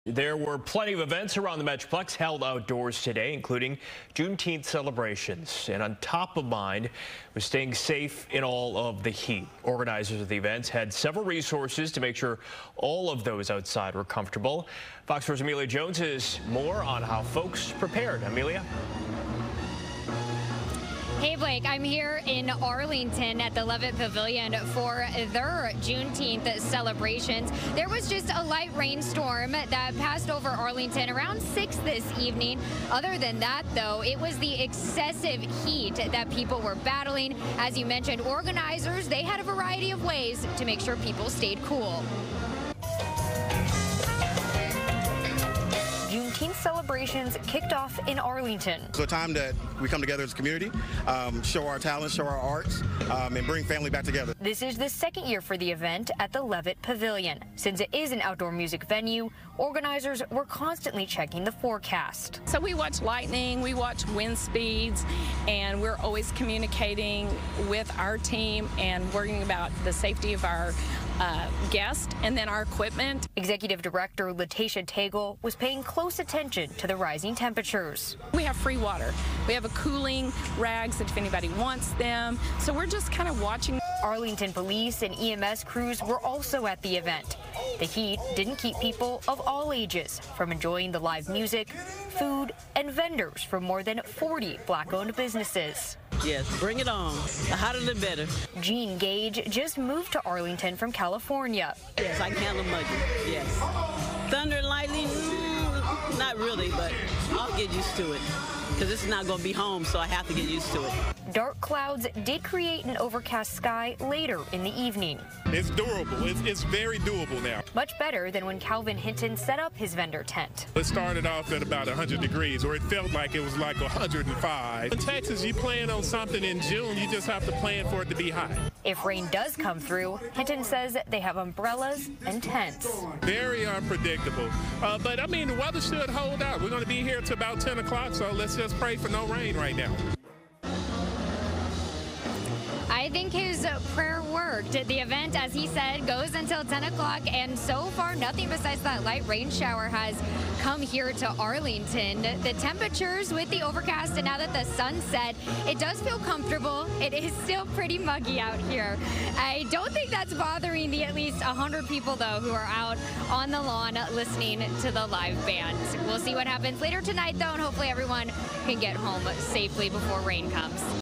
North Texans keeping an eye on the weather for Father's Day, Juneteenth weekend
People were prepared with umbrellas for the rain, but also the excessive heat.